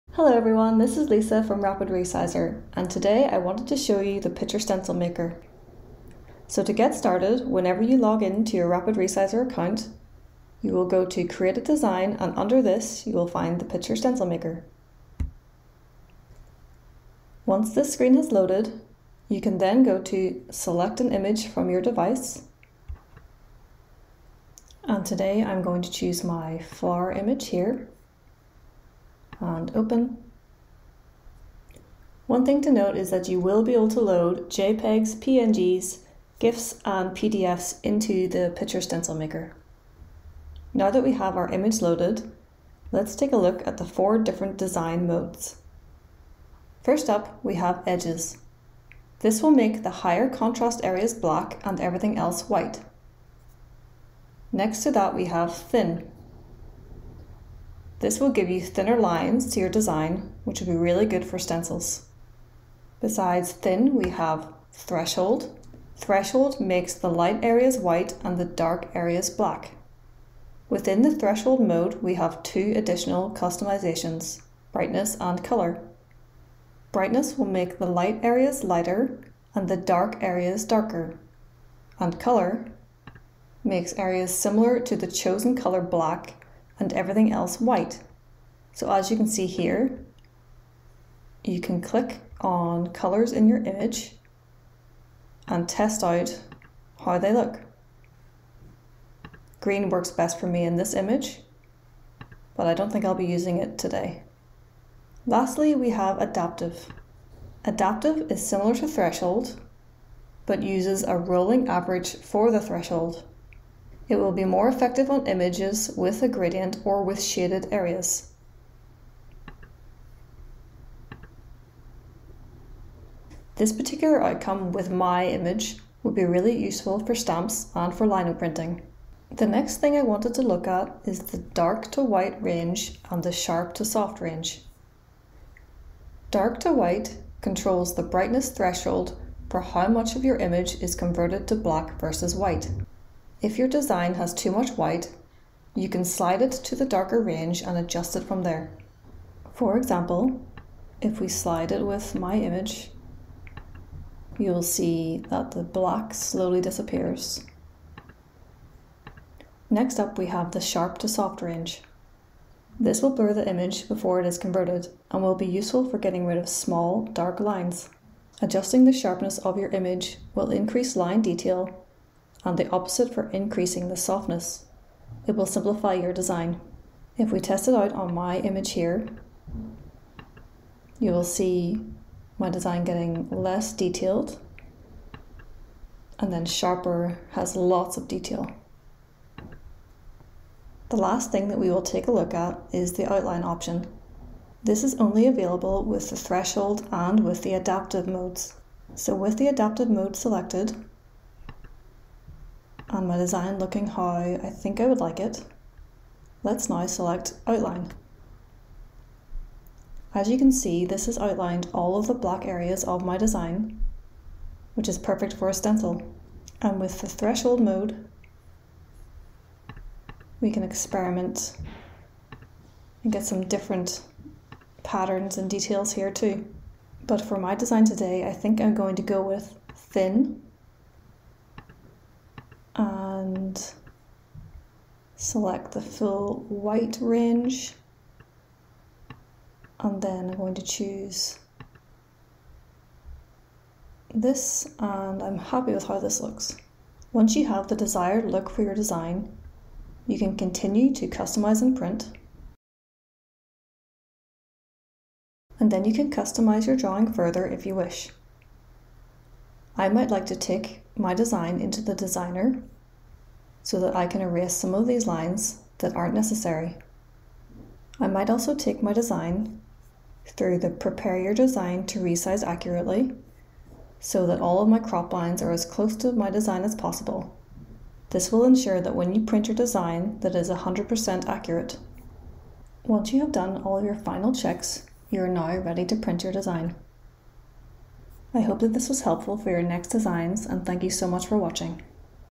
How to use the Picture Stencil Maker
Our Picture Stencil Maker will turn your photographs into black-and-white designs, simplifying them for your next art project.

The Picture Stencil Maker will automatically trace the image you upload into a stencil, pattern, sketch or line drawing while giving you the option to adjust the final outcome.

0:00 Introduction to the Picture Stencil Maker
0:20 Uploading an image
0:42 About the four Design Modes
2:09 About the Dark-White and Sharp-Soft ranges
3:12 Outlining your design